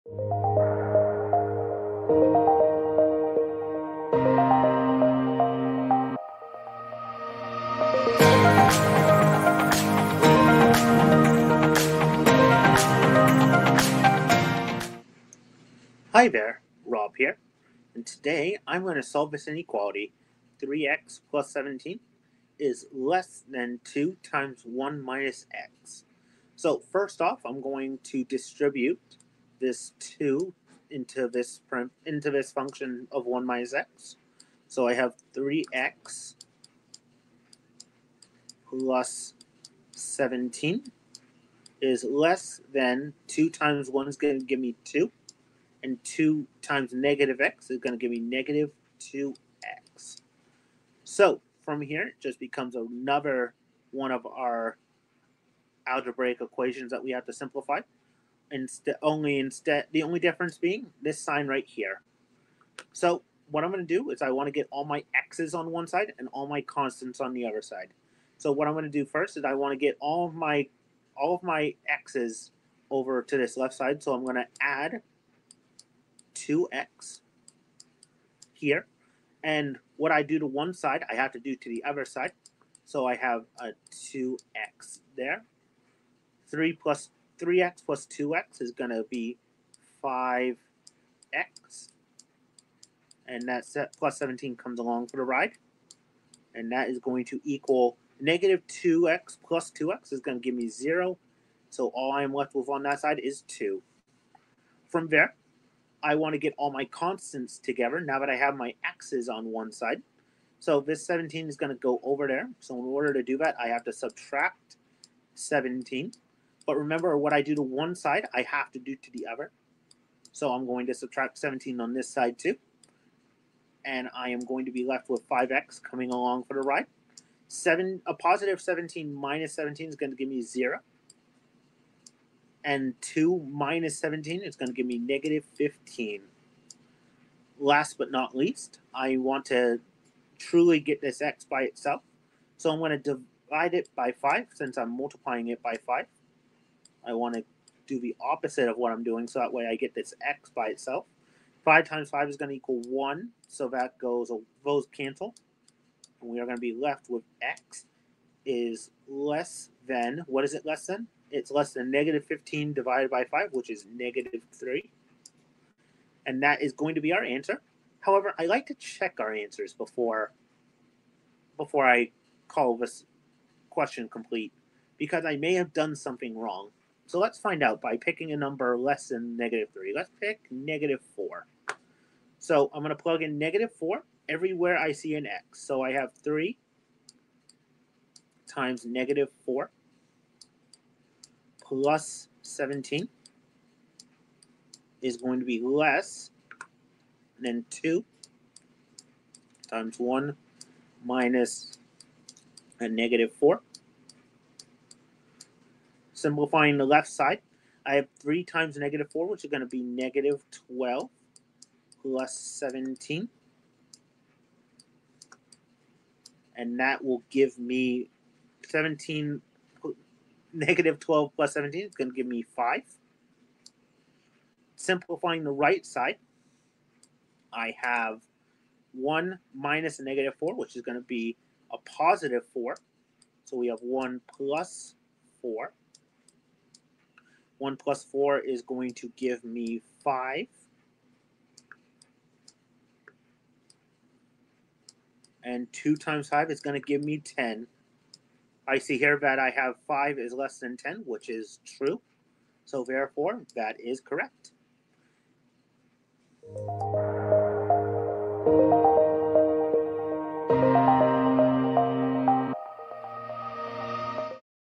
Answer to Nadia's Question
Question: inequality  3x+17‹2(1-x)

Answered By:

Robert T.
Certified Online Math Tutor Specializing in Algebra and Calculus